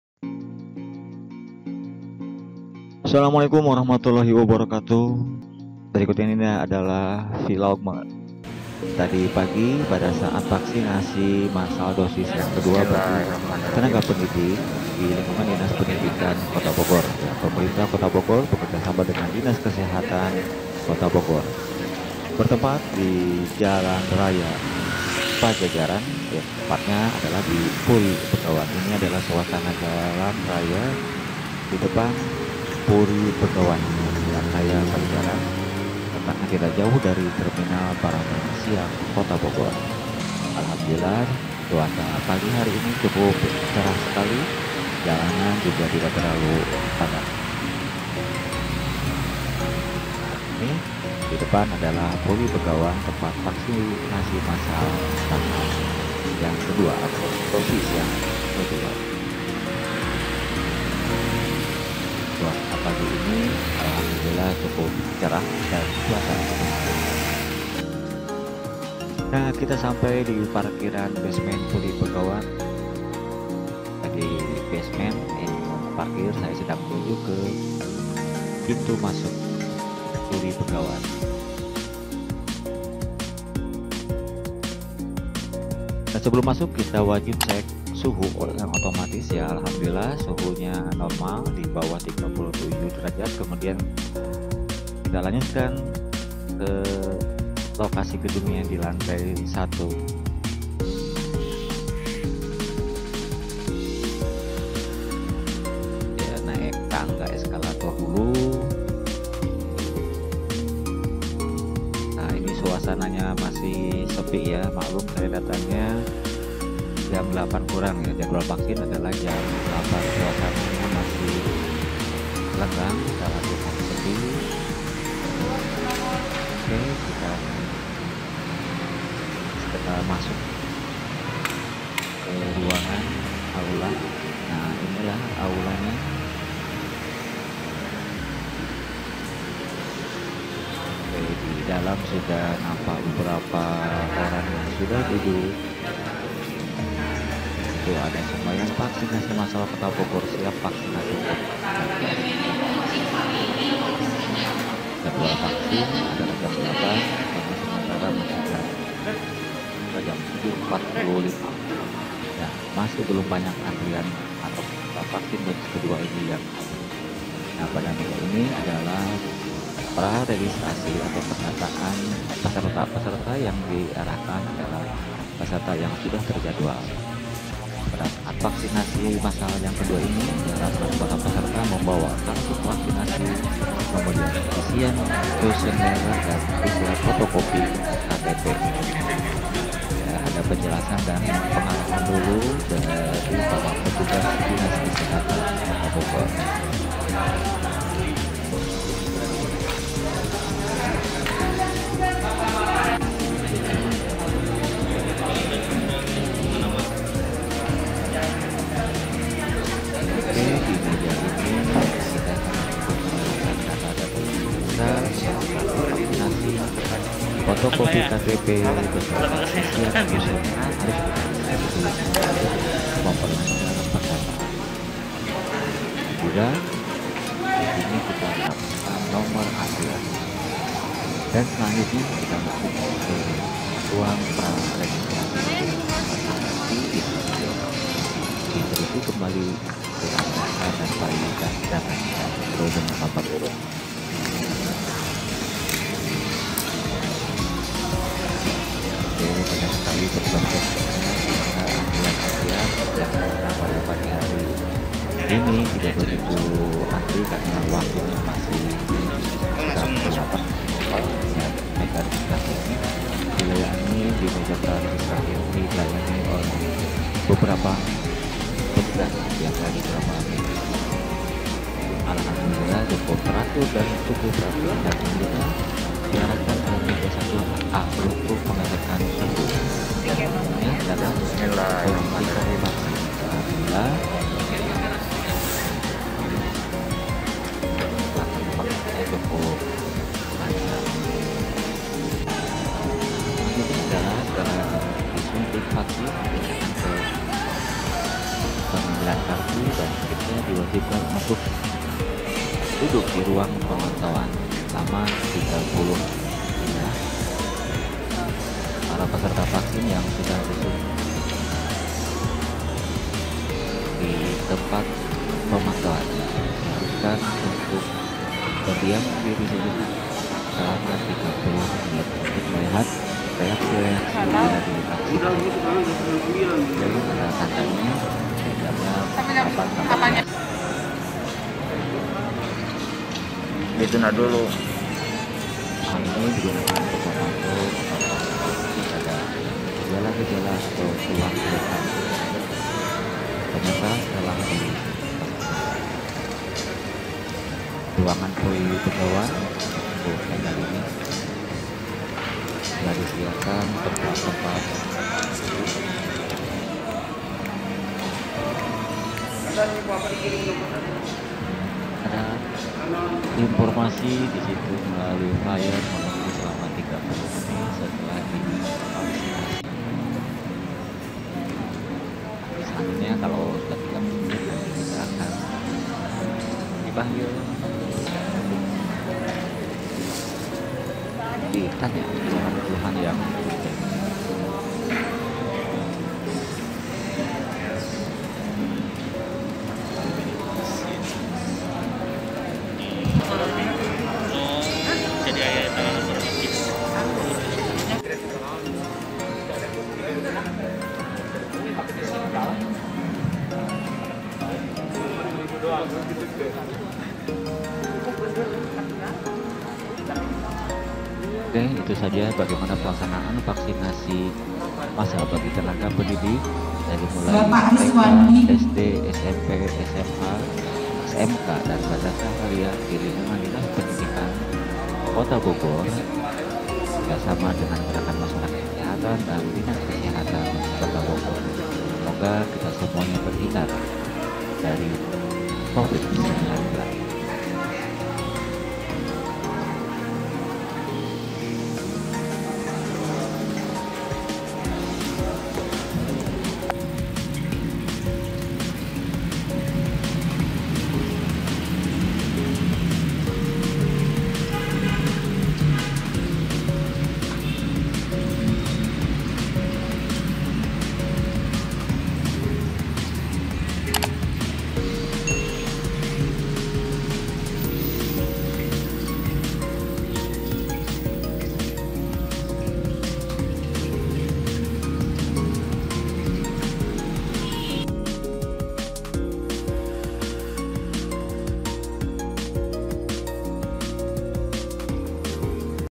Lancar !! Vaksinasi Kedua bagi Pendidik Kota Bogor
Lancar !! Vaksinasi Kedua bagi Pendidik Kota Bogor, dalam menunjang pembelajaran tatap muka tahun pelajaran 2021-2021 pemerintah kota bogor giat melakukan vaksinasi bagi tenaga pendidik dilingkungan Dinas Pendidikan Kota Bogor mulai dari TK, SD, SMP, SMA, MAN dan SMK. bertempat di puri Begawan Jalan Pajajaran Bogor dengan jumlah peserta 3.193 orang dari tgl 31Mei sd 4 juni 2021 orang yang di laksanakan di Puri Begawan Jl. Pajajaran Bogor yang diatur berdasarkan jadwal.
vaksinasi guru,